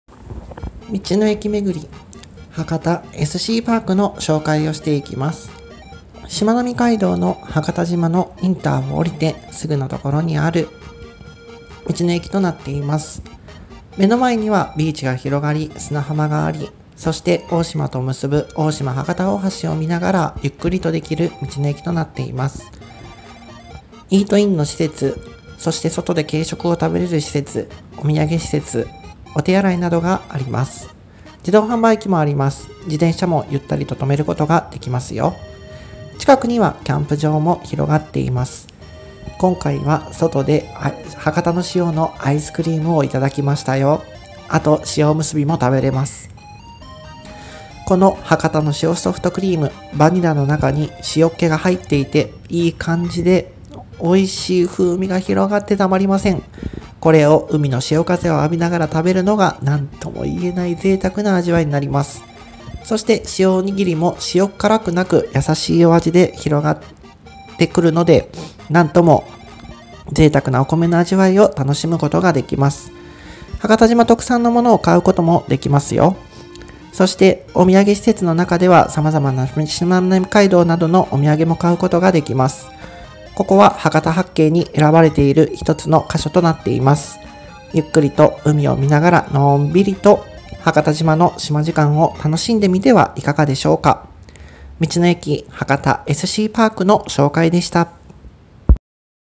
道の駅5愛媛5店舗目伯方scパーク愛媛県今治市 あの塩を味わえる道の駅
道の駅めぐり
道の駅5伯方scパーク
愛媛おいしいものめぐり5伯方の塩ソフト、伯方の塩にぎり
愛媛県今治市

伯方scパーク
駐車場、WC、レストラン、お土産、軽食、海
伯方島インター下りてすぐにある道の駅。目の前には伯方大島大橋を臨むビーチが広がる。伯方の塩を使ったグルメを楽しめる。

今回は外の軽食コーナーで伯方の塩ソフトとおにぎりをいただいた
伯方の塩にぎりは、おむすびで温かいごはんに焼き塩チックにかかったものをいただける。ほどよい塩辛さがたまらない。後にソフトクリーム。バニラベースに伯方の塩が混じり甘塩っぱい爽やかな味わいを楽しめます。海を見ながら食べるのおすすめです。

しまなみ海道のお土産が揃ったり伯方の特産品が買えたりします。海を見ながらのんびりしにいってみませんか